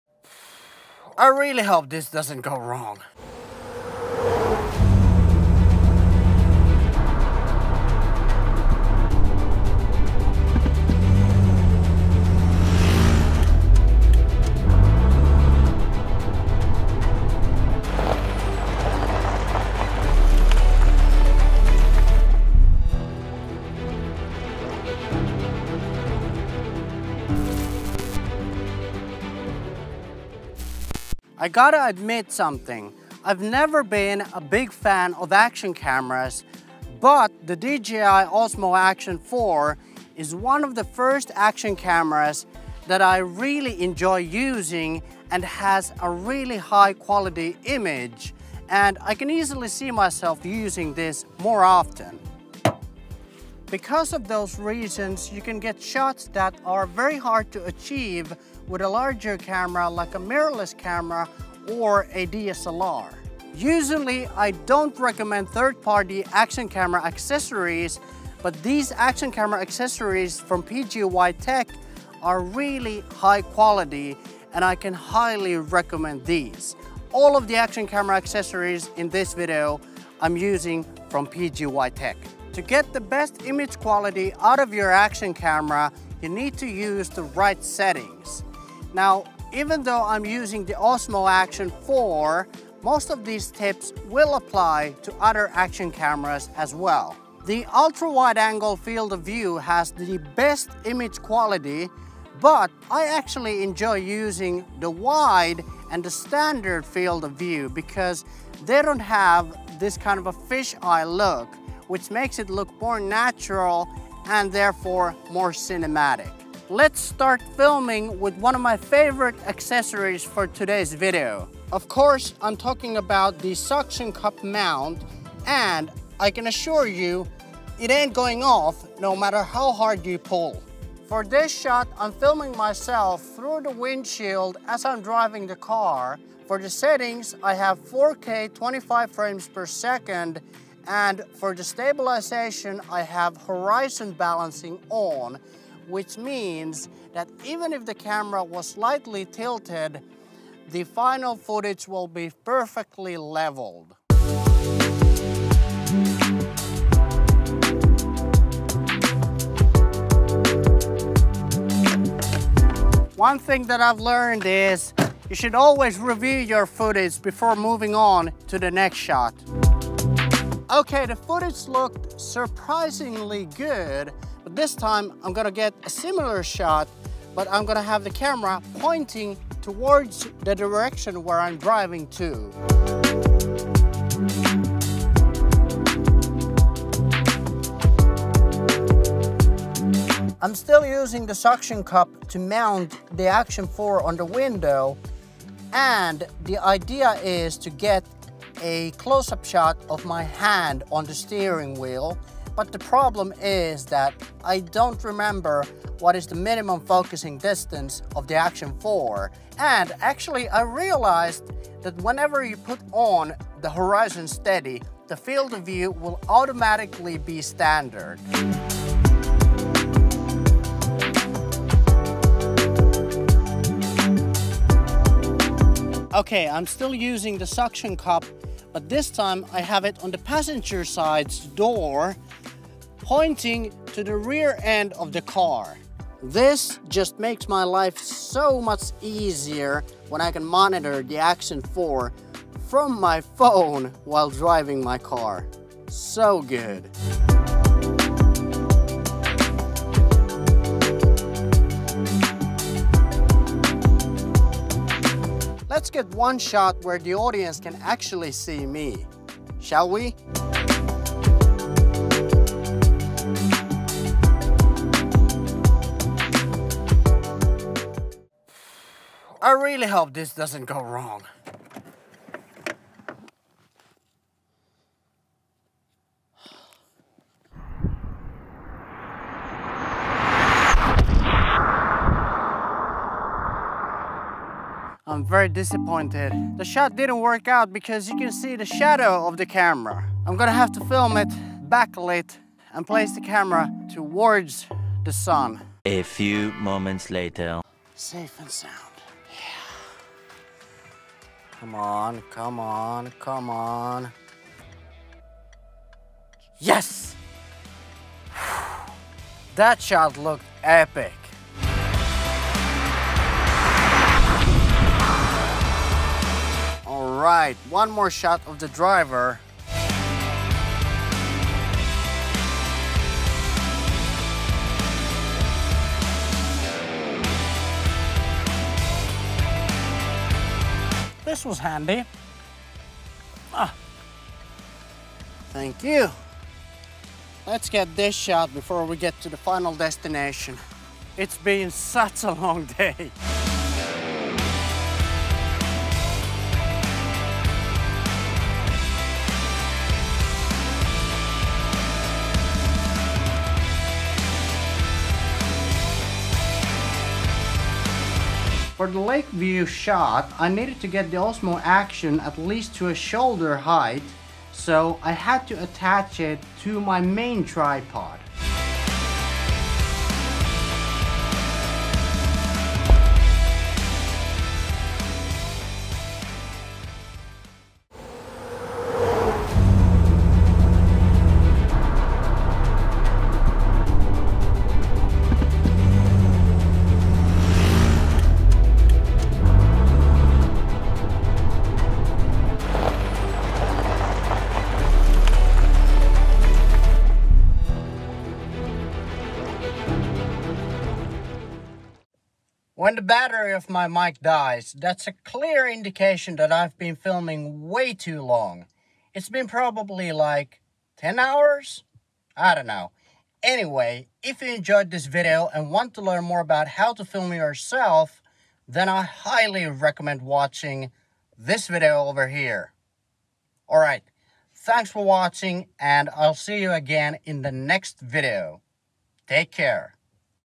How to Film a Cinematic Car Video by Yourself | DJI Osmo Action 4
Here's how to film a cinematic car video by yourself with the DJI Action 4! In this video you will learn how to use the right setting cinematic setting with an action camera. I will also teach how to film yourself and show the behind the scenes of making this video.

⚡️ ALL MUSIC & SFX FROM ARTLIST